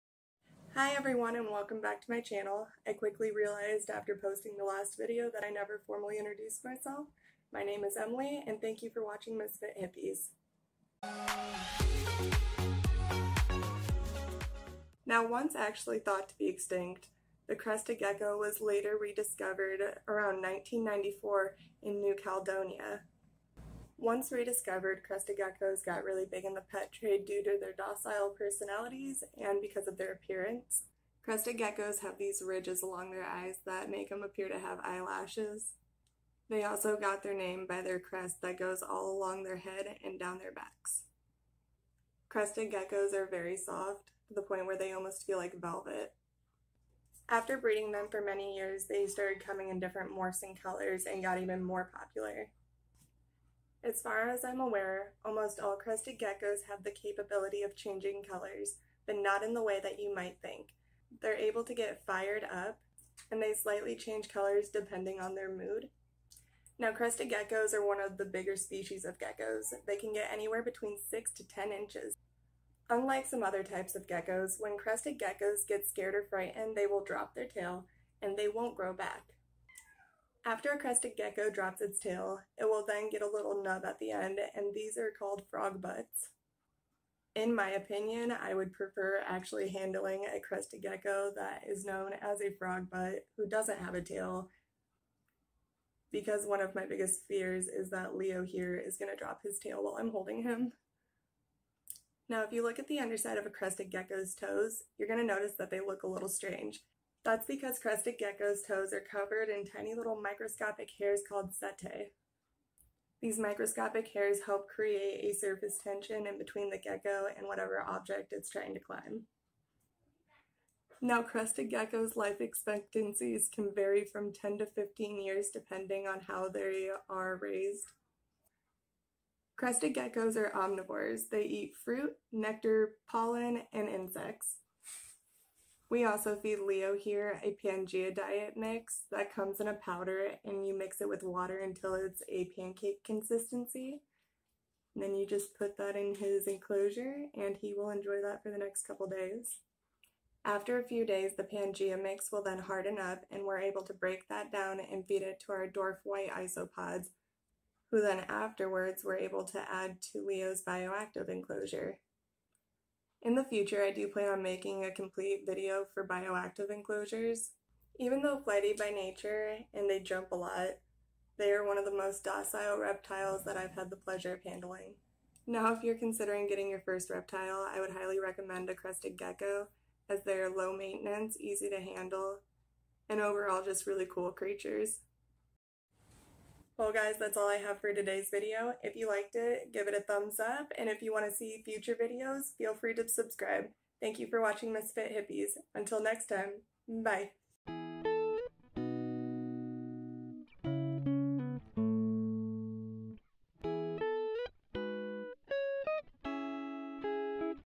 Crested geckos 101
With the help of Leo, I would like to teach you a few things about crested geckos.